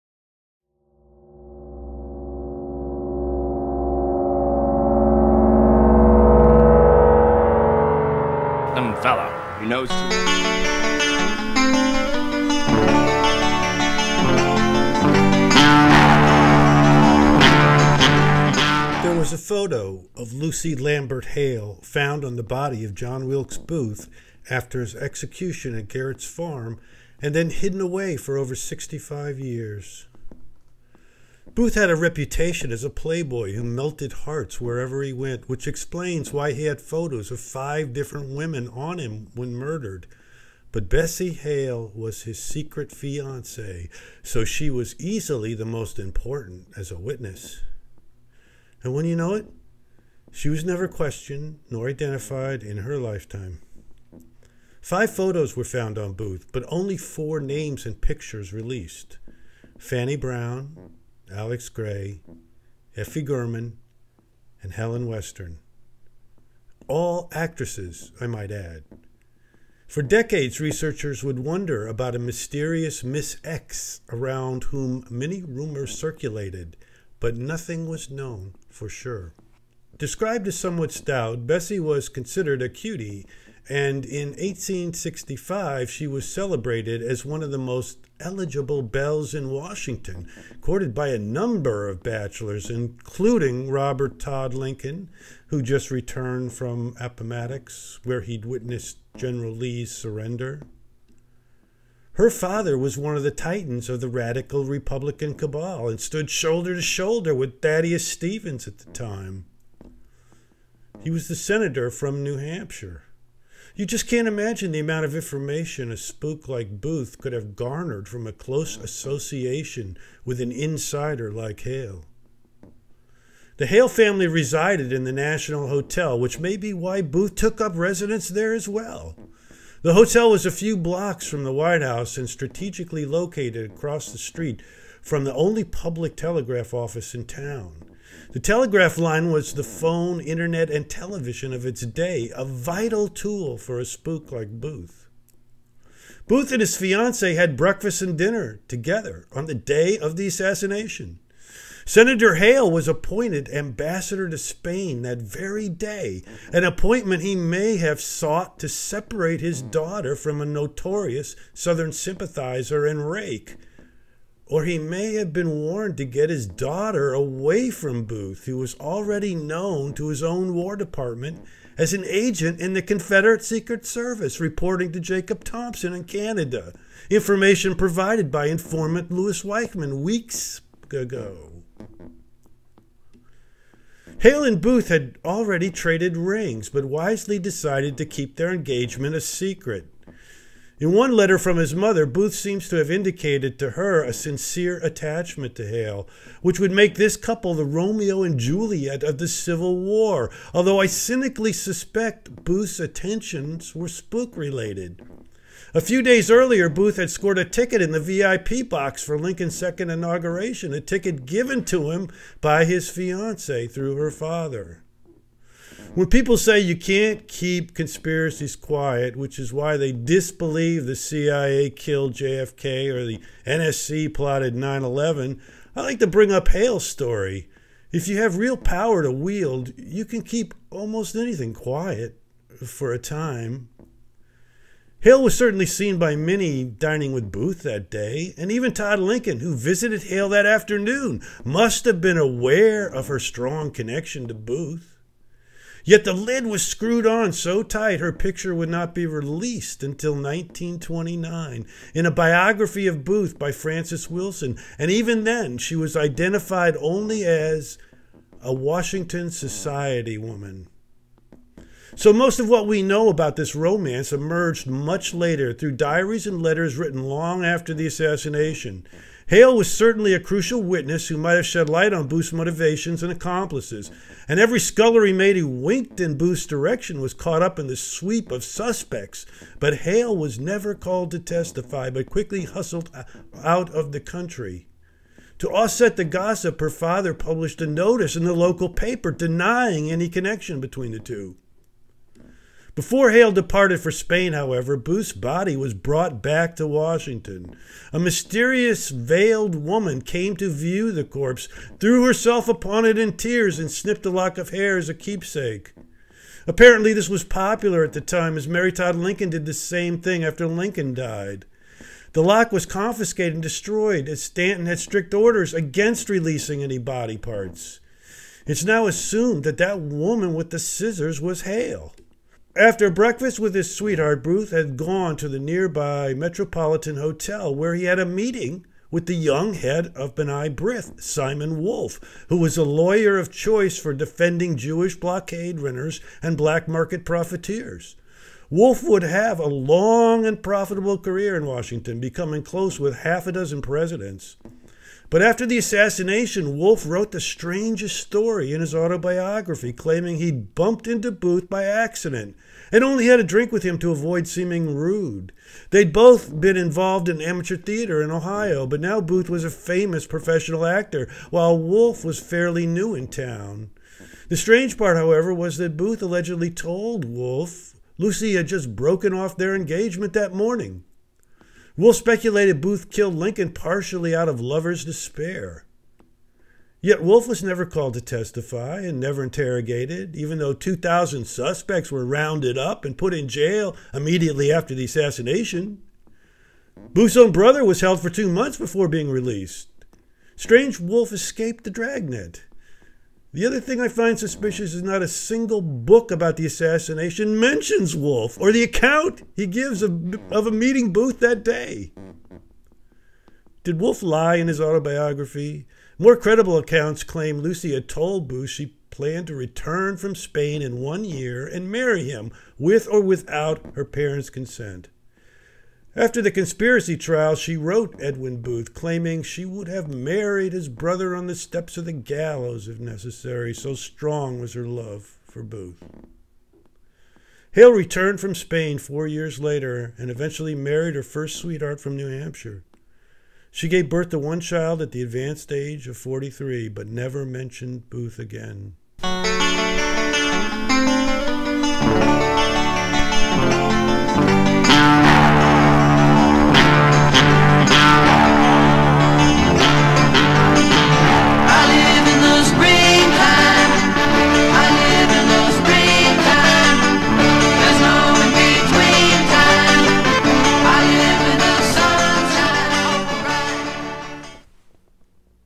The secret loves of John Wilkes Booth
Why did it take so long to reveal the identity of one of Booth's female friends?